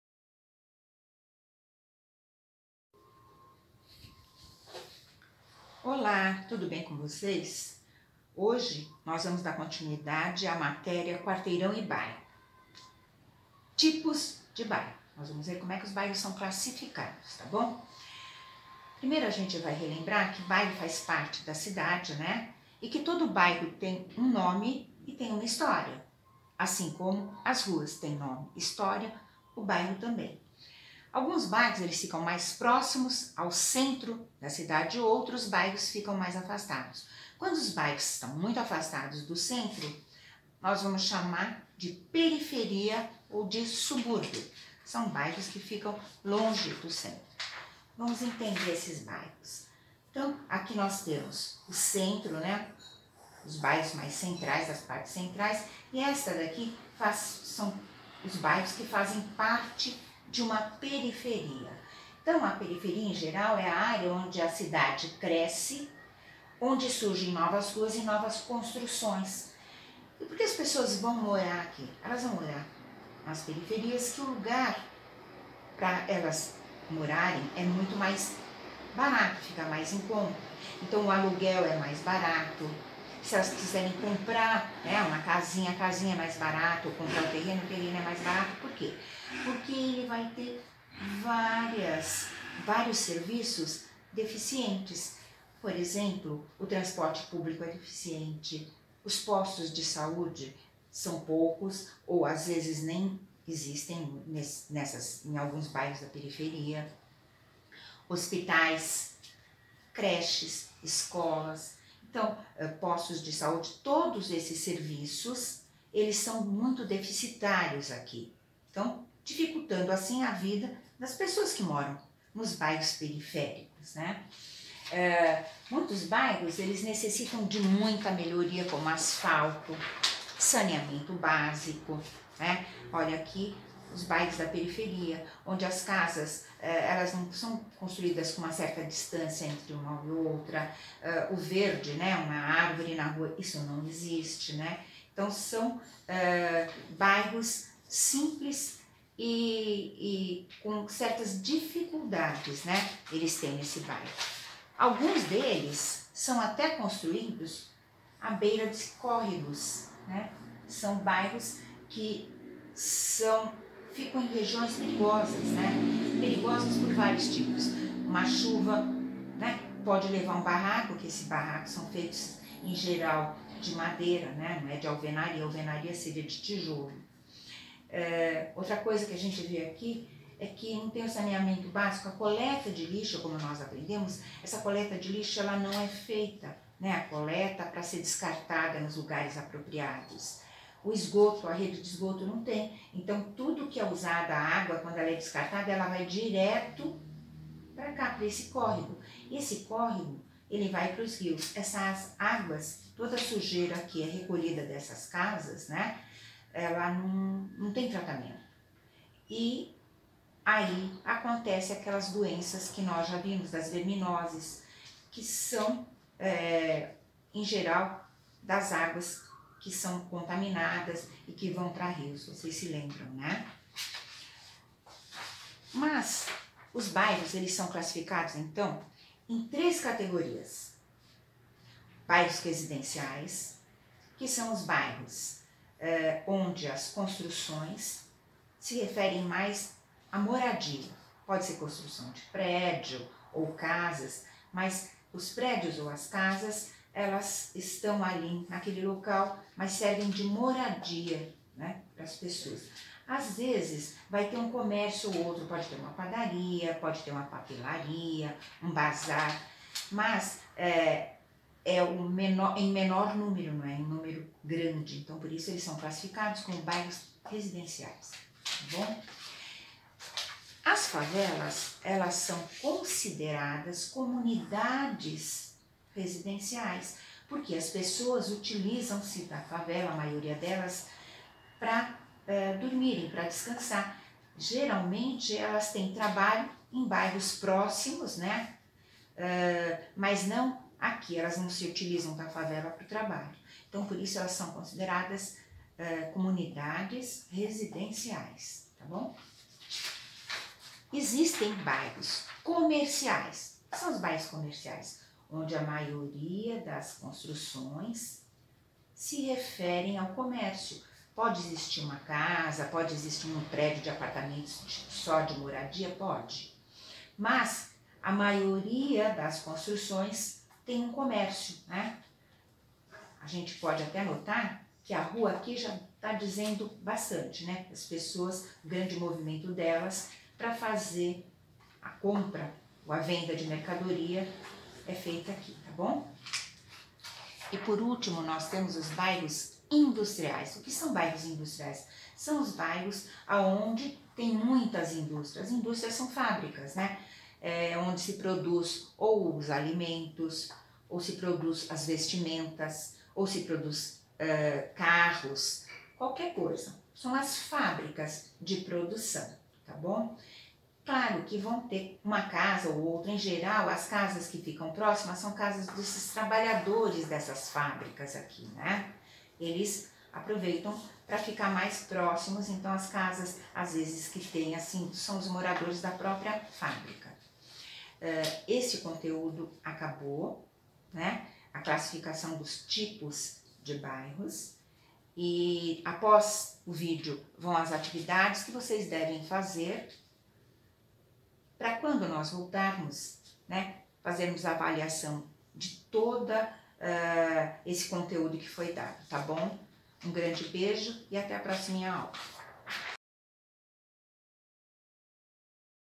Tia Christina explicando Geografia: tipos de bairros - 24/06/2020
Vídeo para as crianças do 3º ano